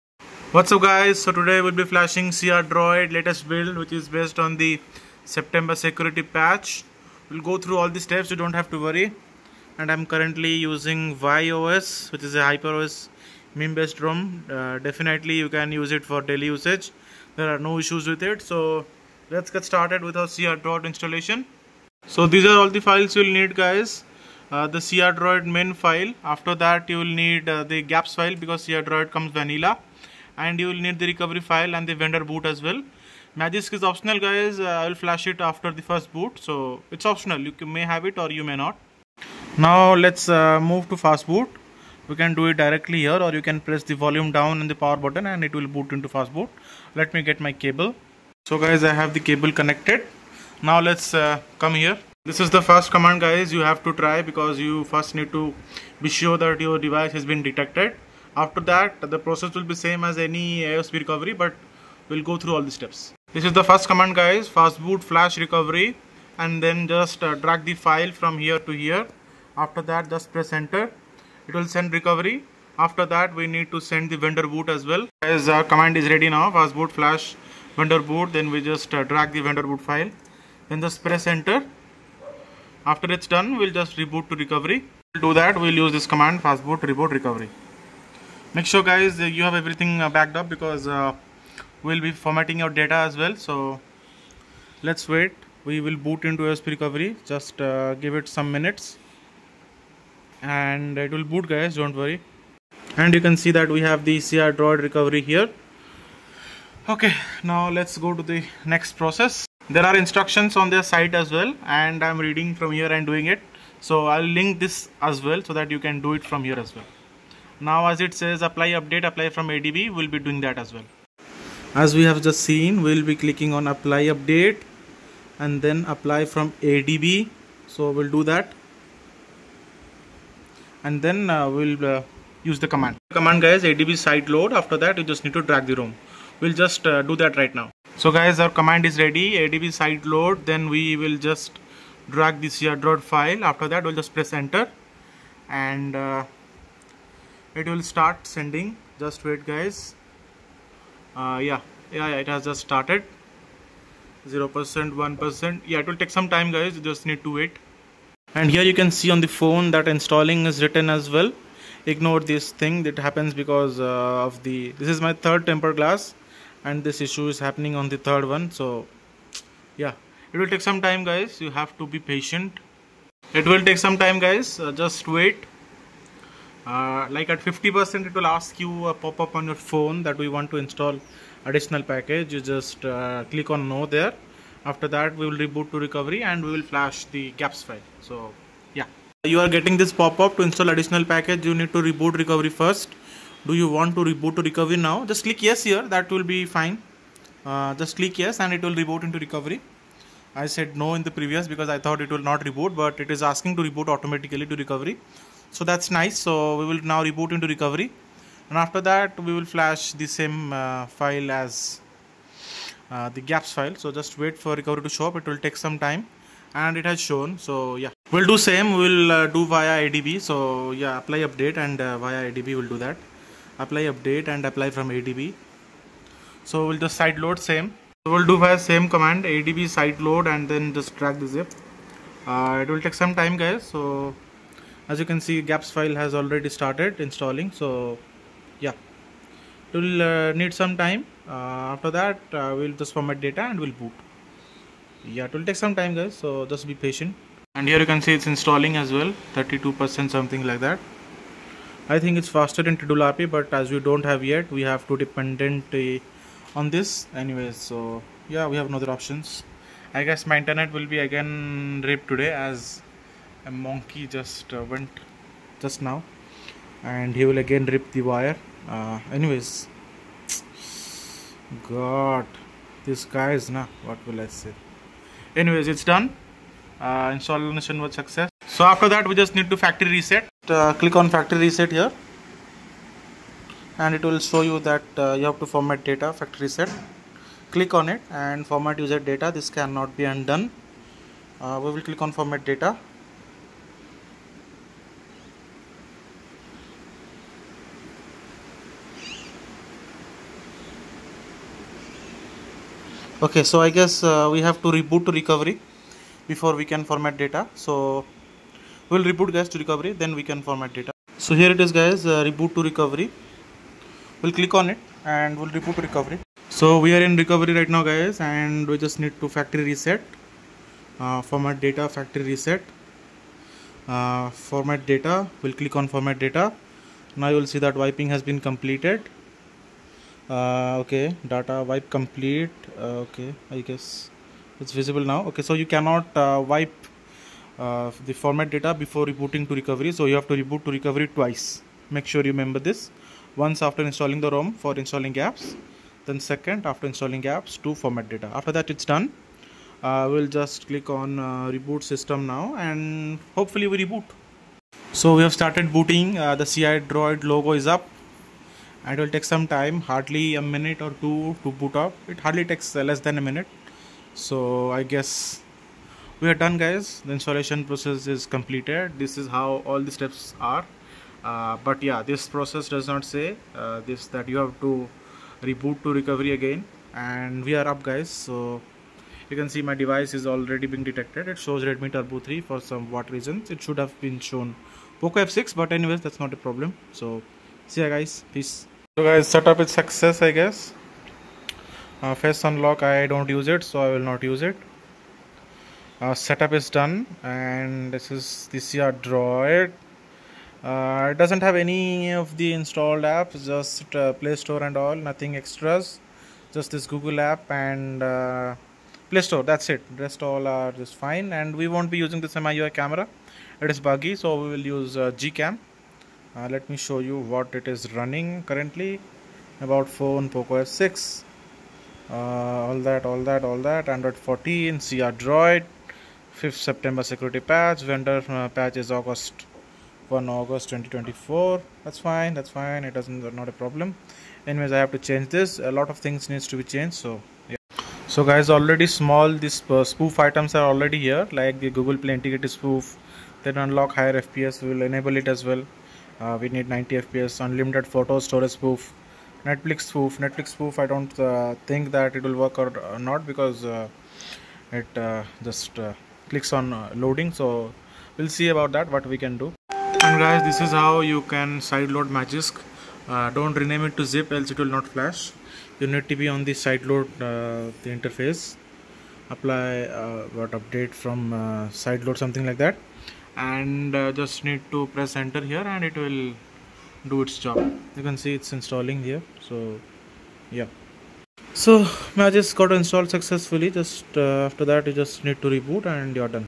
How to Install CrDroid 10.8 on Poco F6: A Complete Step-by-Step Guide
Unlock the full potential of your Poco F6 with CrDroid 10.8! In this comprehensive guide, we’ll walk you through each step of the installation process—from unlocking the bootloader and flashing a custom recovery to successfully installing CrDroid 10.8. Whether you're new to custom ROMs or a seasoned pro, this guide simplifies everything you need to know, ensuring a smooth and hassle-free experience. Get ready to enhance your phone’s performance, battery life, and features with CrDroid's powerful customization options!

CrDroid 10.8 - OFFICIAL | Android 14.
Updated: 14/09/24